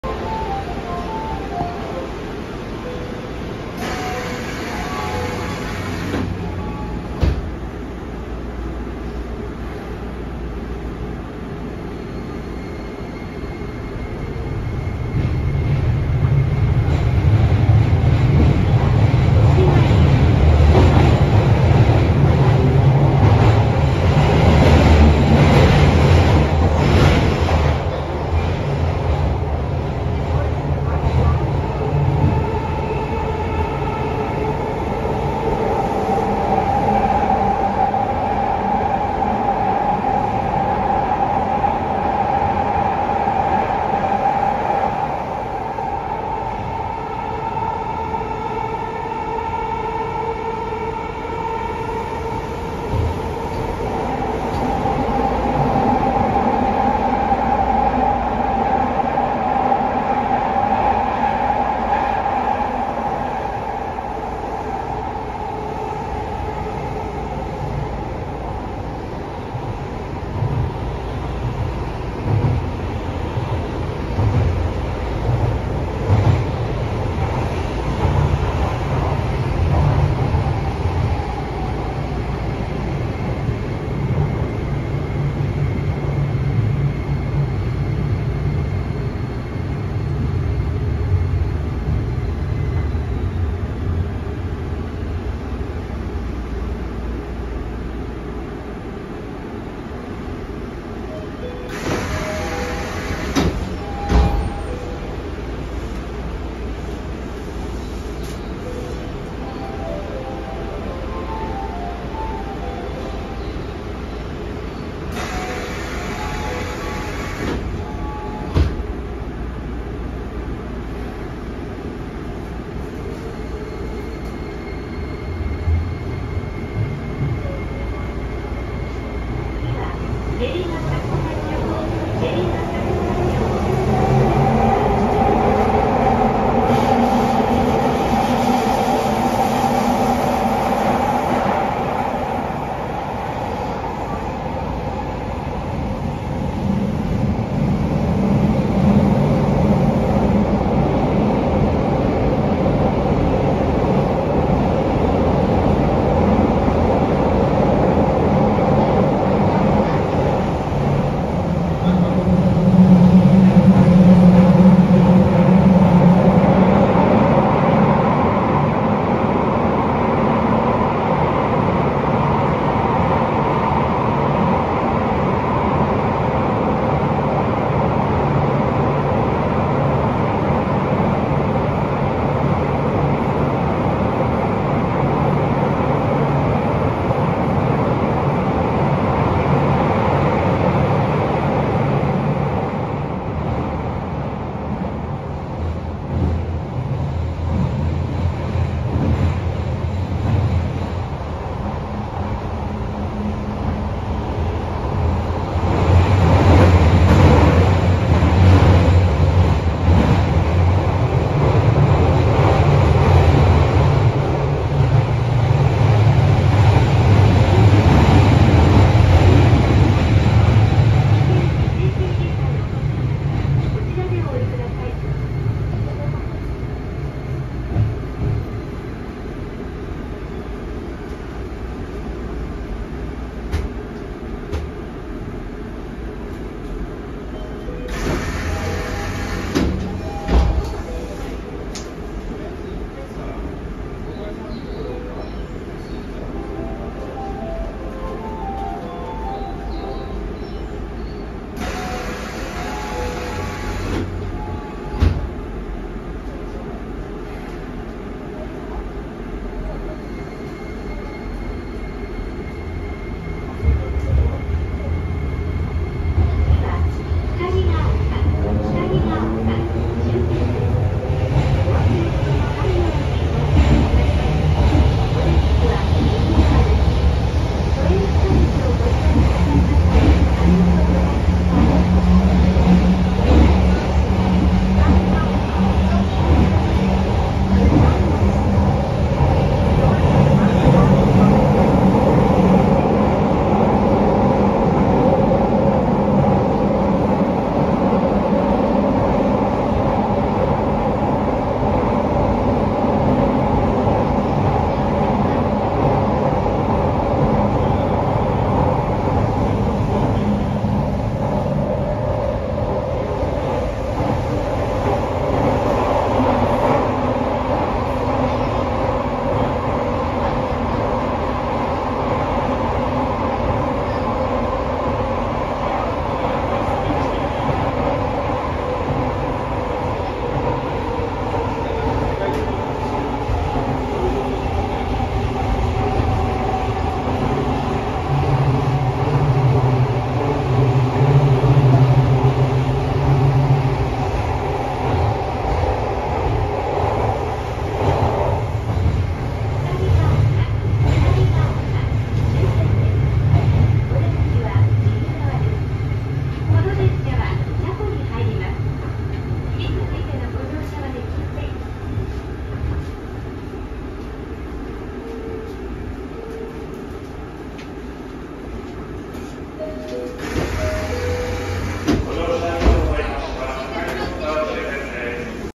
【日立SiC】都営大江戸線12-600形12-811F 走行音・車内 (練馬→光が丘)
12-801Fからドアチャイムの音程が低くなりました。これはこれで好きですね。